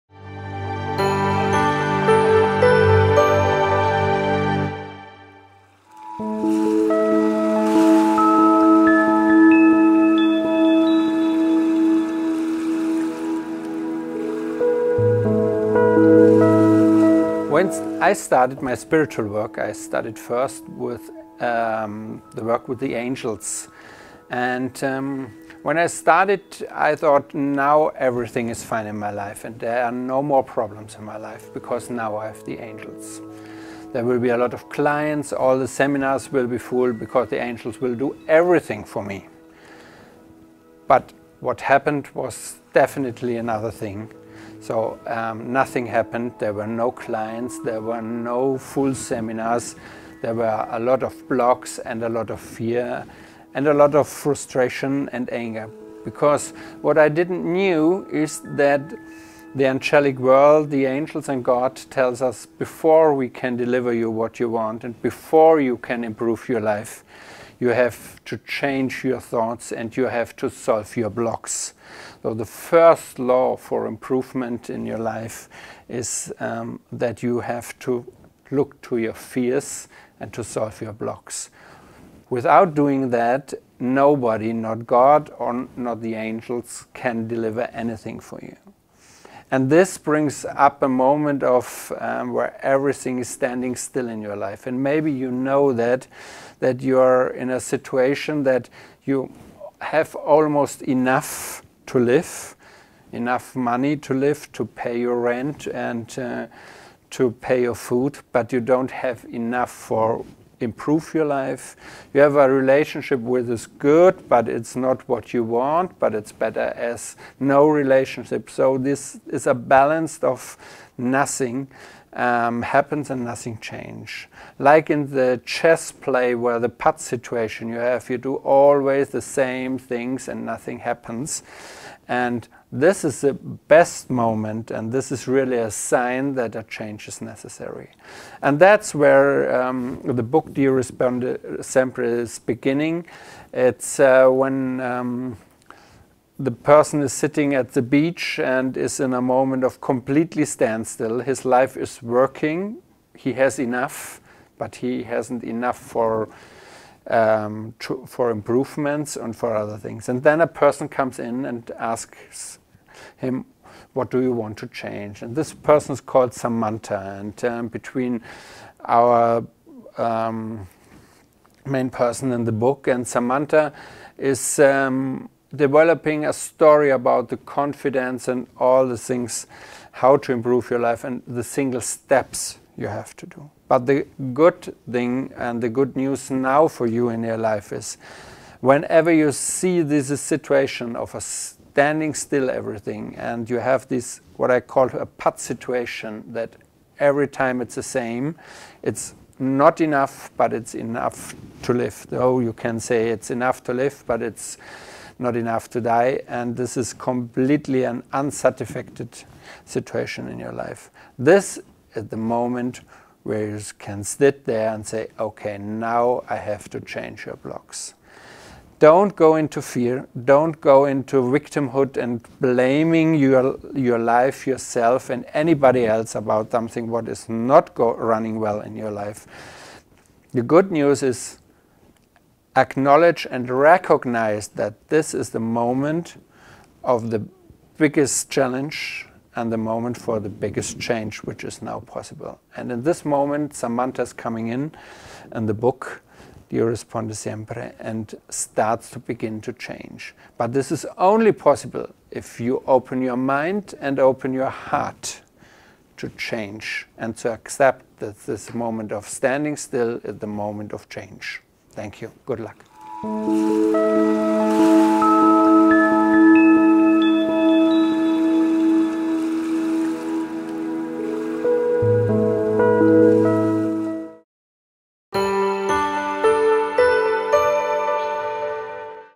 4. Il momento del cambiamento - Hubert Kölsch
- Hubert Kölsch, autore di "Dio risponde sempre" (Anima Edizioni) ci accompagna per 12 video alla scoperta del significato della fiducia e di come scoprire il “linguaggio di Dio” - Quarta puntata.

Per trovare tutti i video di Hubert, vista la sua pagina su nonsoloanima.tv a questo link:

Le entità angeliche e Dio si aspettano, prima di poterci dare quello che desideriamo o migliorare la nostra vita, che cambiamo il nostro modo di pensare e sciogliamo i nostri blocchi.

La prima regola per migliorare la nostra vita è guardare in faccia le nostre paure e risolvere i nostri blocchi. Se non lo facciamo, nessuno, né Dio né gli angeli, potrà aiutarci.

La buona notizia è che quando vedete una situazione di stallo e siete insoddisfatti, questo è il momento perfetto per sedersi e dire a se stessi: «Adesso devo cambiare qualcosa rispetto ai miei blocchi».

Non cedete alla paura e al vittimismo, non incolpate la vostra vita, voi stessi o chiunque altro per ciò che non sta andando bene, ma siate consapevoli e riconoscete che questo è il momento della sfida più grande e del più grande cambiamento possibile.

Aprite la vostra mente e il vostro cuore al cambiamento, e accettate che il momento di stallo diventi il momento del cambiamento.

... e nella COMMUNITY Google+

I commenti che non rispettano la Netiquette potrebbero essere rimossi.